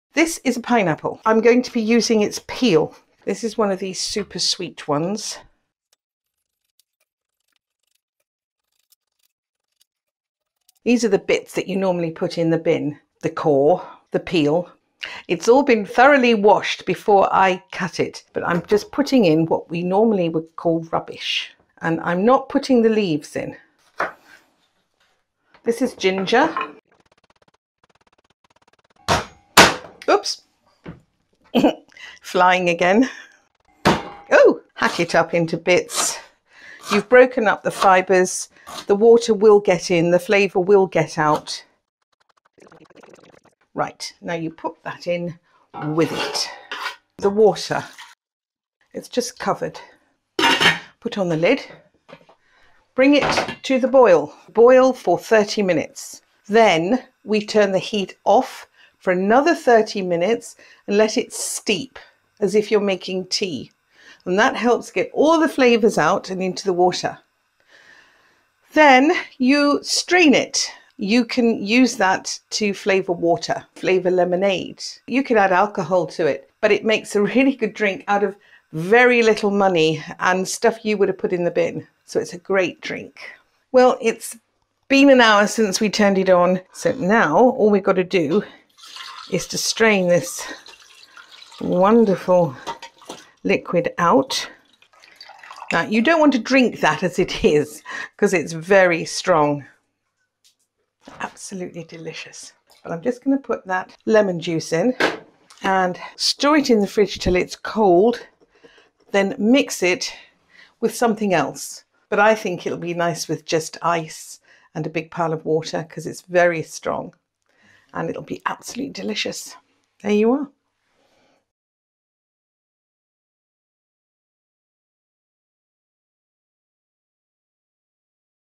pineapple ginger juice drink
A delicious drink made with ginger and leftover pineapple, a perfect cordial as a mixer for lemonade, or with just water and ice.